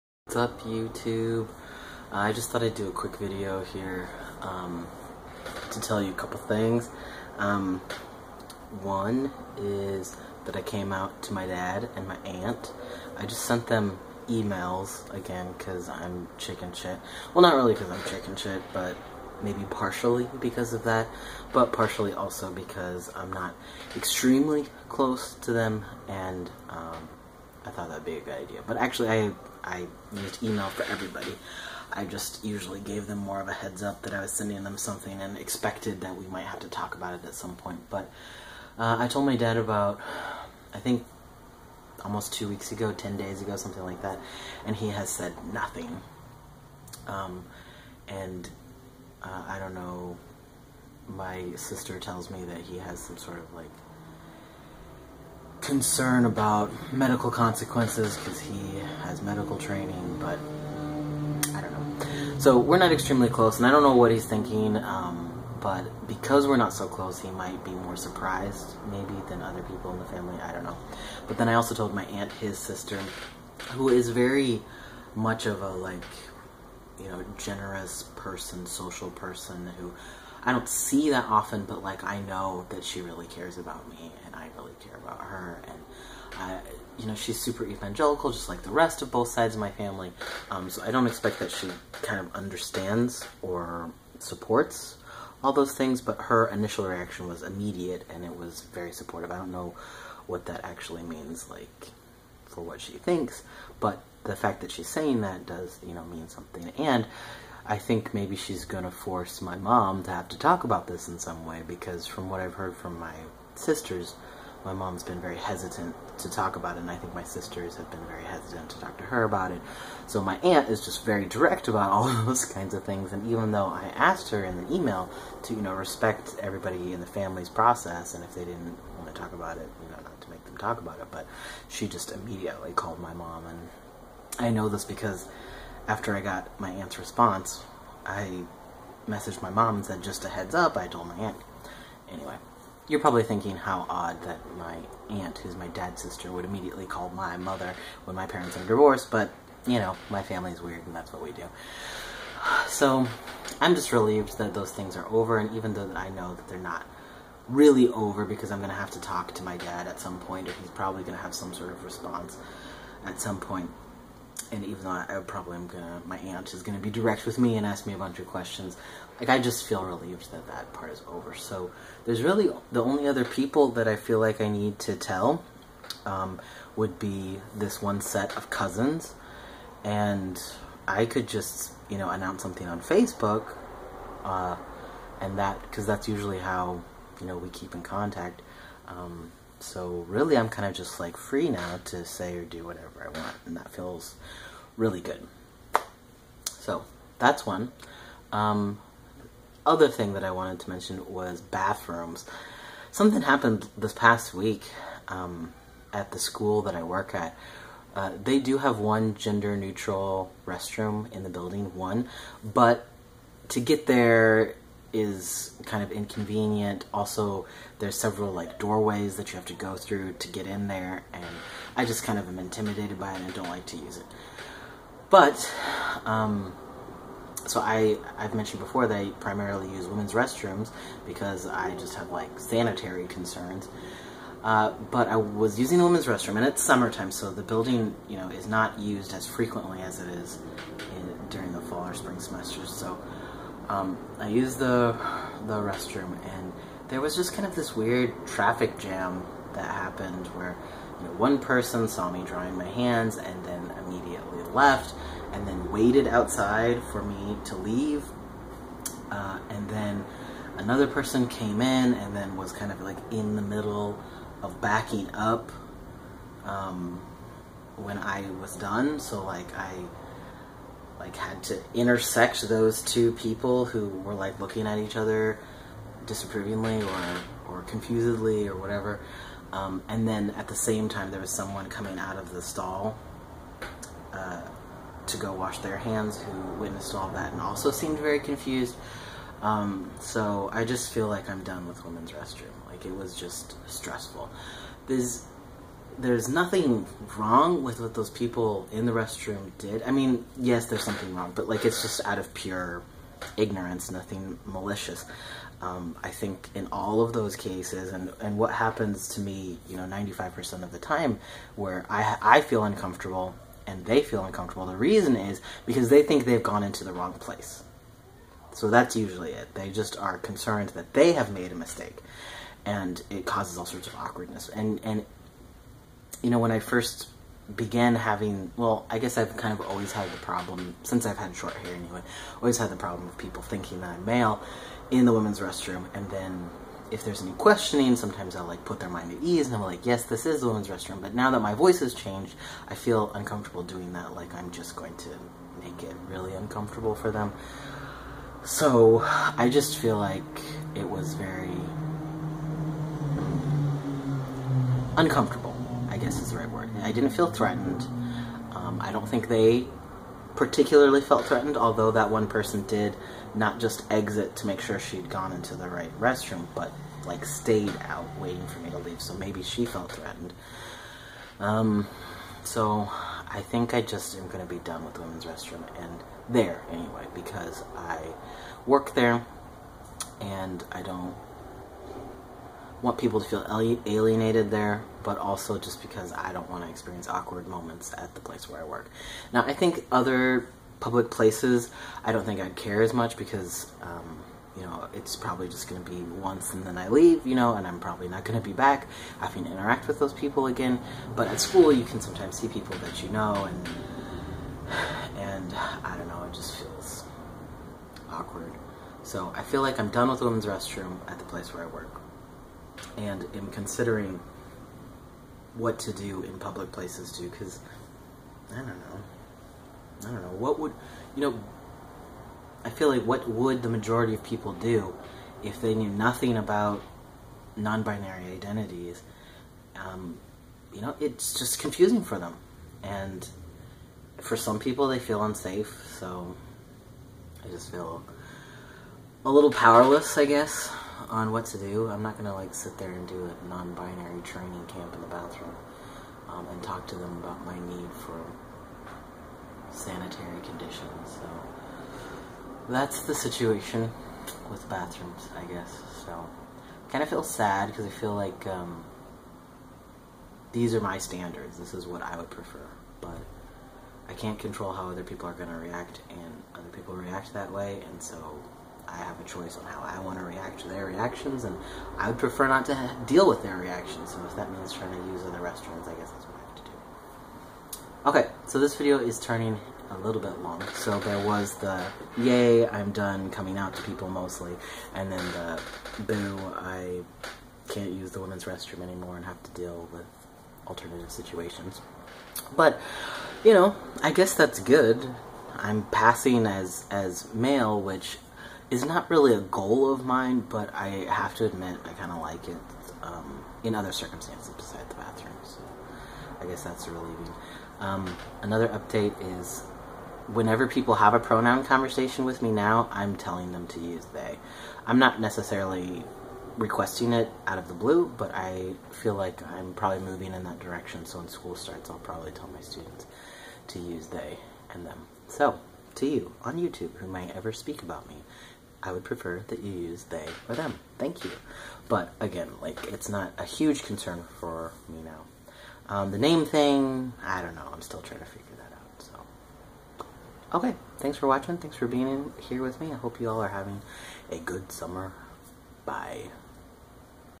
More coming out, bathrooms, pronouns [Nonbinary, 7 months on T]
The first half of this video is me talking about coming out to my dad and aunt. The second half is me talking about how I feel like I'm done using the women's restroom at work. And the last 30 seconds is me talking about changing my pronoun preference: they/them please and thanks!
About me: I'm a nonbinary trans FTM-ish POC documenting my experiences and the latest transition in my life: HRT.